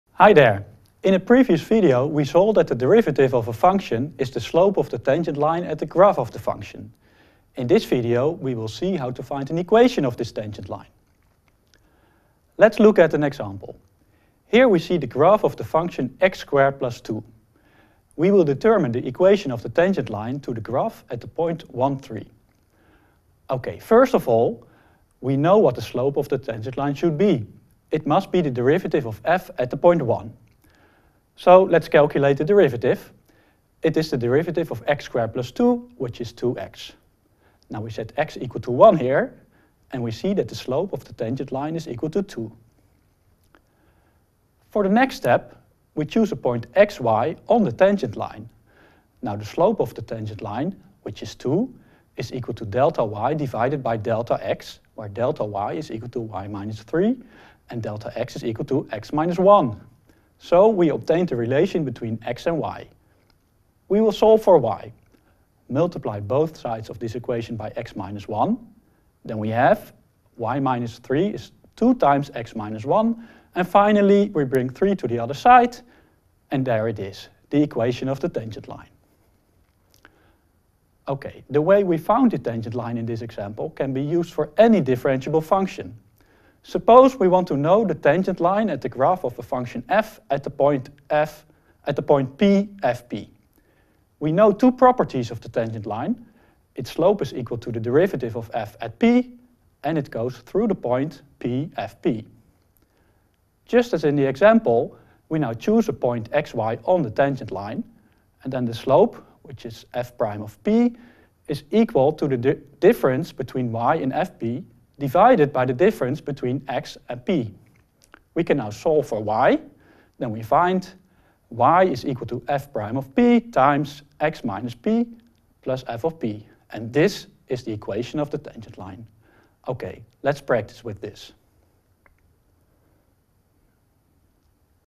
Tangent line - Differentiation - Mathematics - Pre-university Calculus - TU Delft
This educational video is part of the course Pre-University Calculus, available for free ©️ TU Delft, released under a CC BY NC SA license The derivative of a function is the slope of the tangent line at the graph of the function. In this video we will see how to find an equation of this tangent line. This video is part of the edX course: pre-university Calculus (Calc001x)